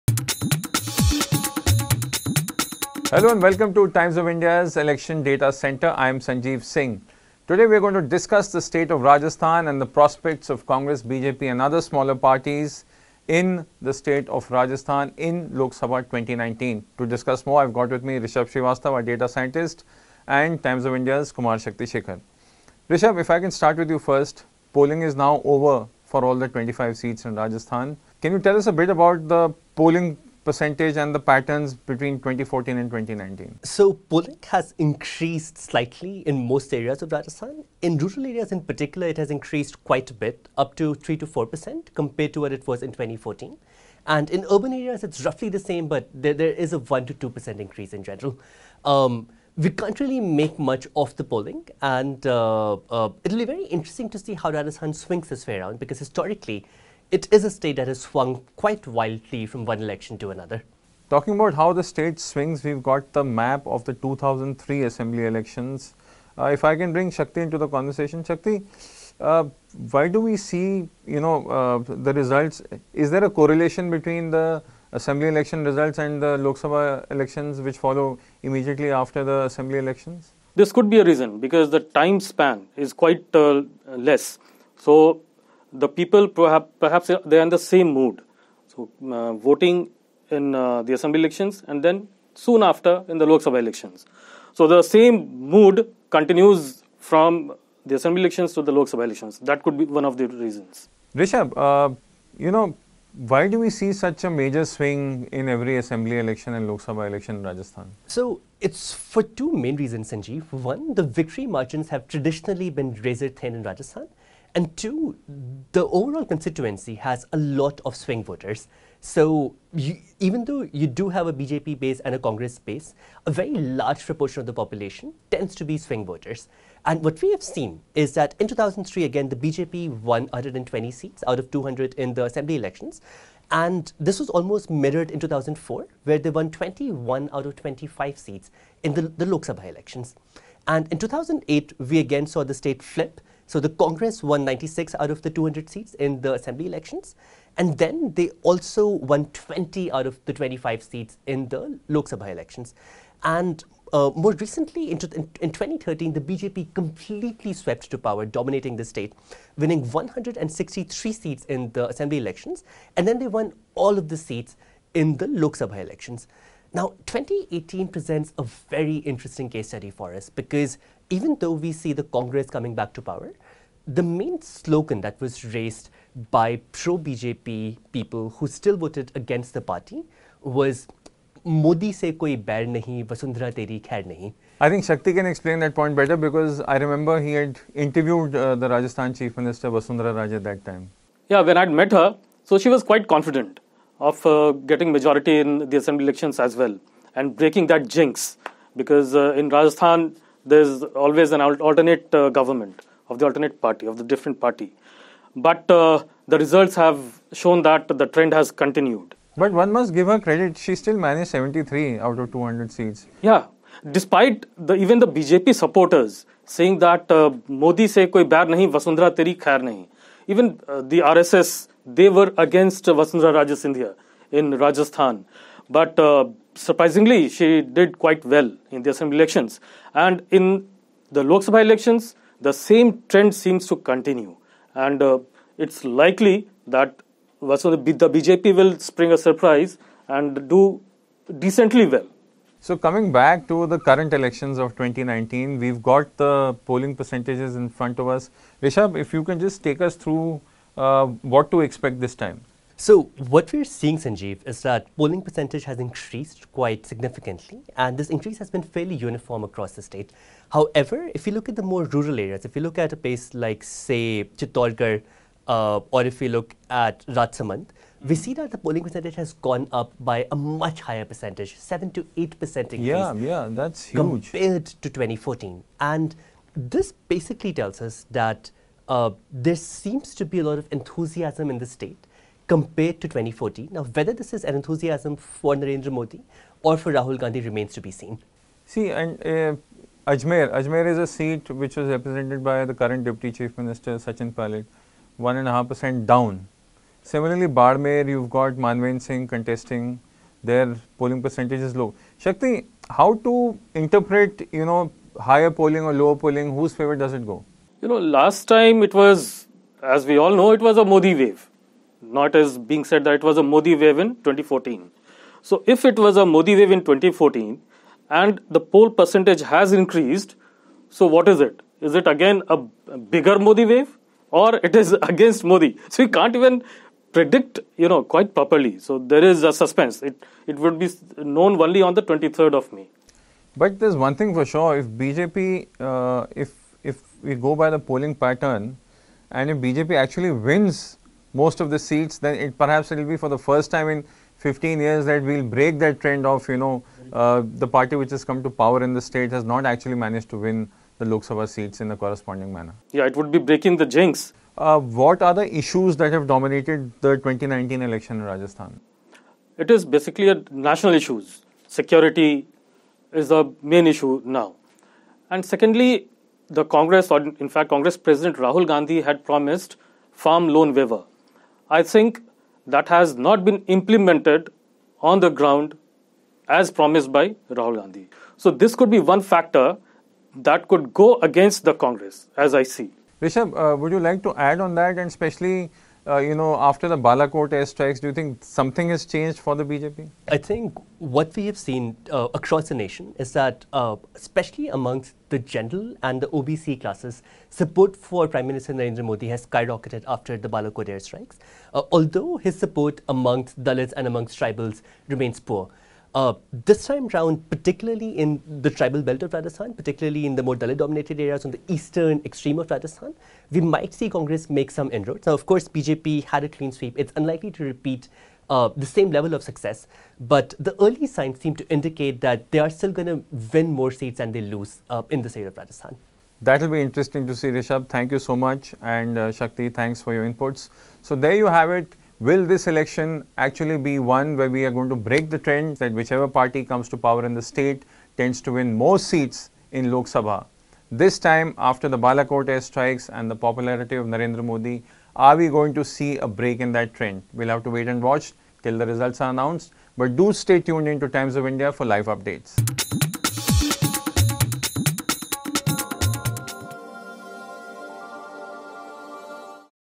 Can Congress counter the Narendra Modi factor in Rajasthan?
It was the only state where Congress felt confident it would return to power in the assembly elections held in November-December 2018.